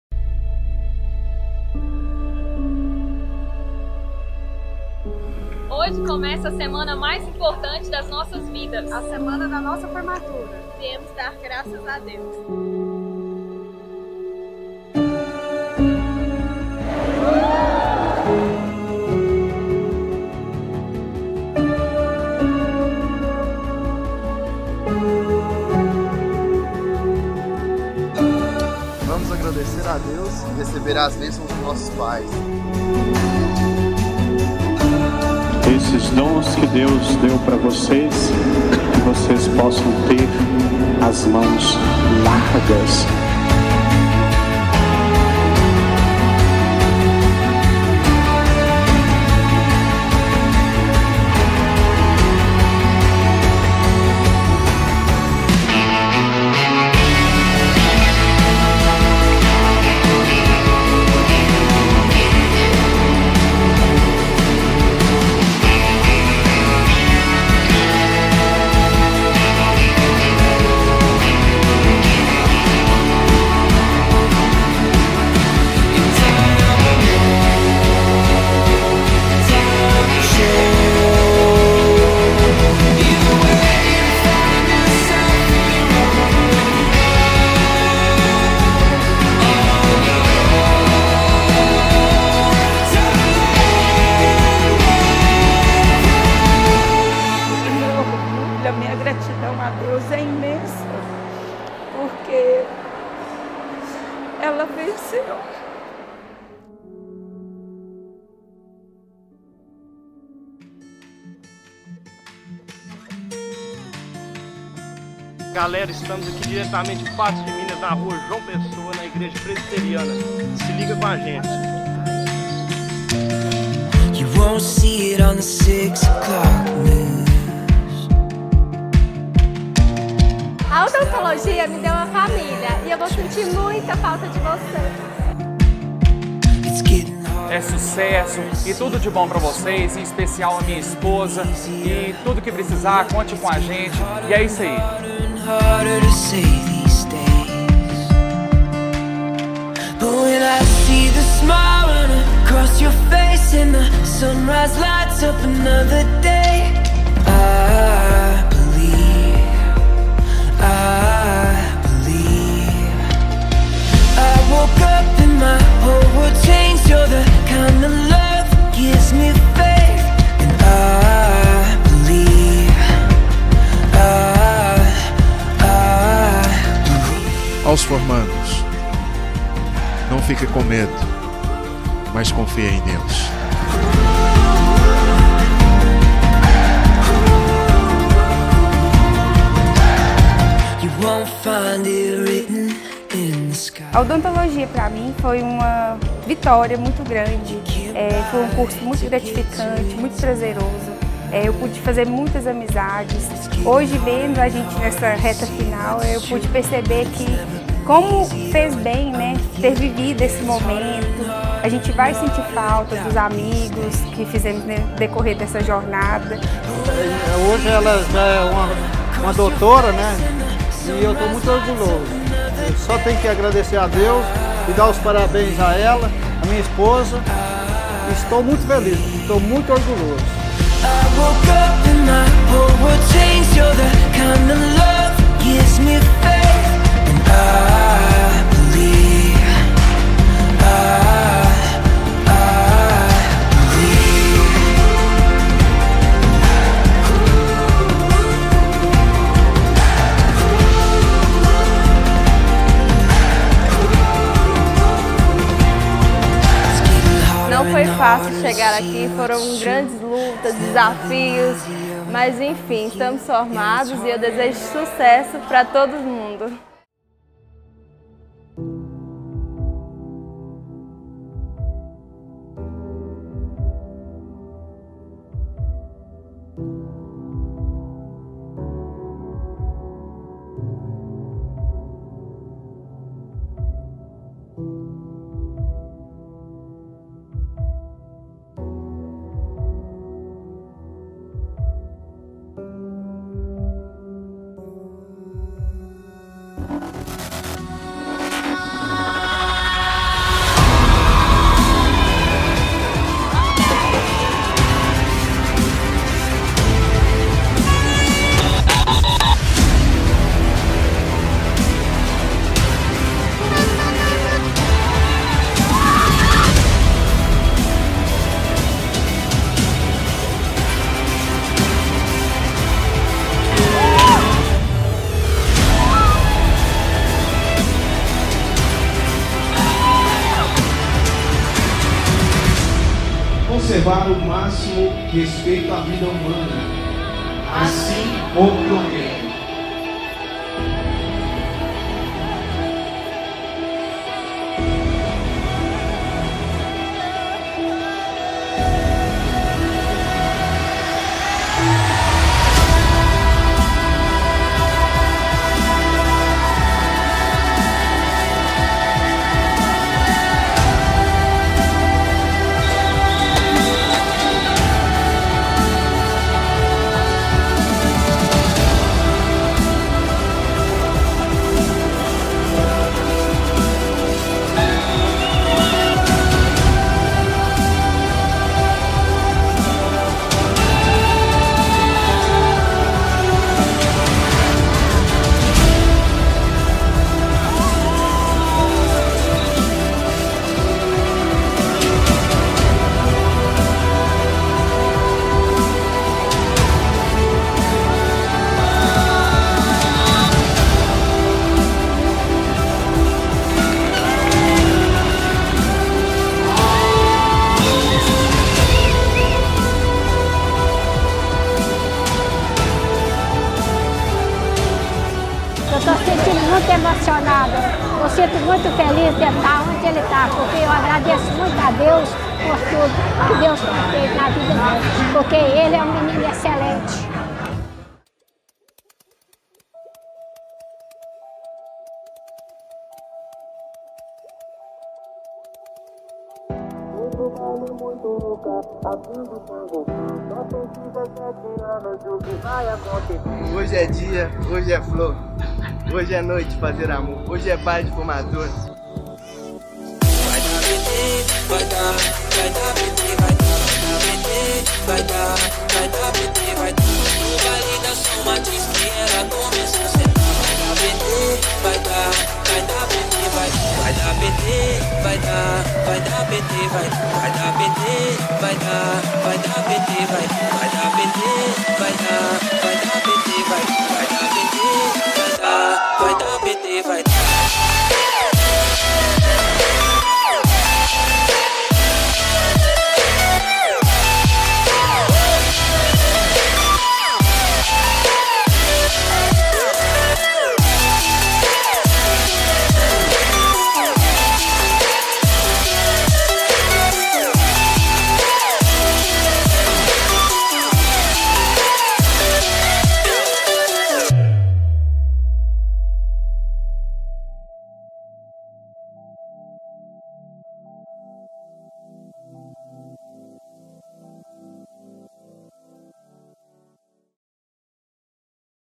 FORMATURA DE ODONTOLOGIA - SAME DAY - PATO DE MINAS | Status Formaturas
Duvido você não se emocionar com este show de imagens da Status Formaturas no nosso TBT de hoje.

A música é boa, as pessoas são lindas, a formatura está bombando e os formandos e as famílias transbordando de emoção diante da realização, da conquista...após anos de jornada e muito sacrifício!

Obrigado aos formandos de Odontologia da Faculdade de Patos de Minas, por nos dar a oportunidade de registrar estes momentos únicos em vossas vidas.
Para nós, cada formatura é ímpar, mas esta é uma das inesquecíveis!

Status Formaturas. Sua conquista merece ter status.